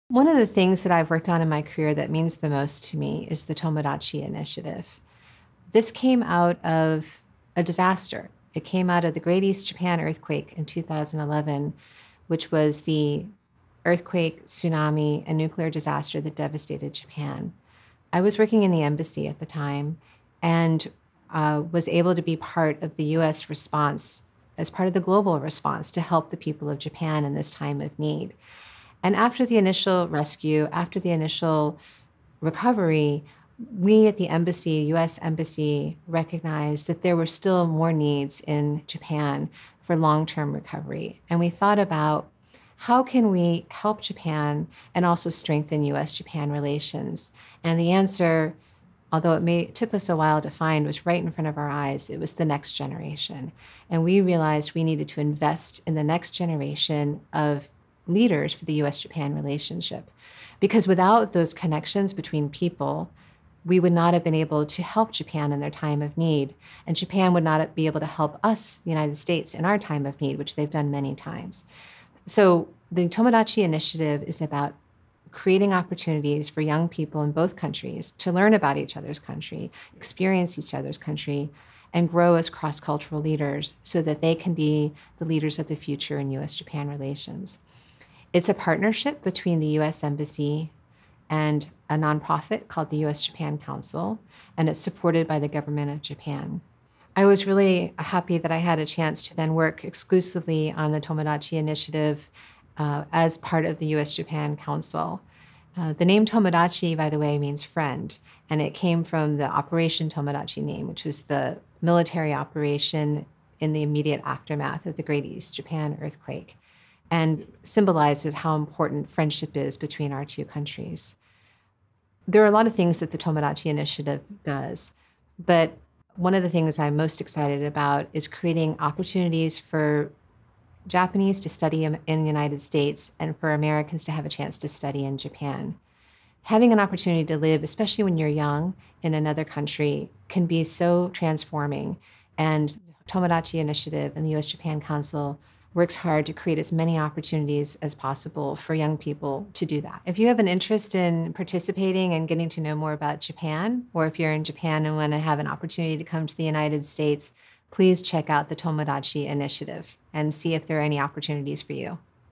The TOMODACHI Initiative | Chief of Staff Suzanne Basalla | Career Girls
Watch Suzanne Basalla, Chief of Staff at Toyota Research Institute. She and her team work on developing new technologies and support systems for older adults to improve their quality of life.